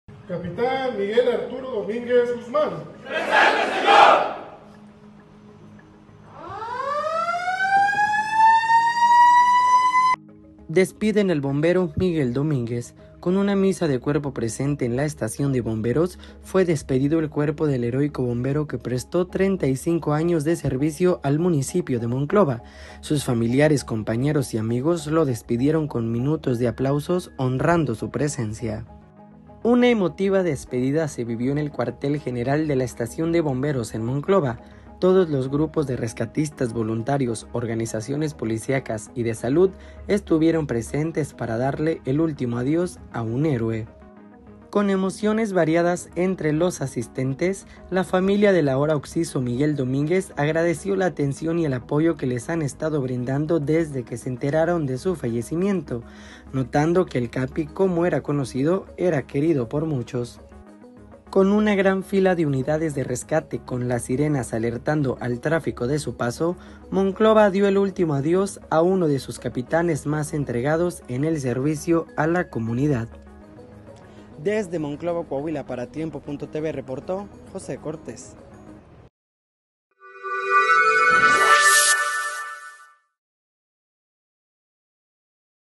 Despedida emotiva al bombero Miguel Domínguez en Monclova
En una ceremonia conmovedora, familiares, amigos y colegas dieron el último adiós al Capitán Miguel Domínguez, quien dedicó 35 años al servicio de Monclova. La misa de cuerpo presente en la estación de bomberos fue un homenaje lleno de emoción y reconocimiento. Un verdadero héroe que siempre será recordado.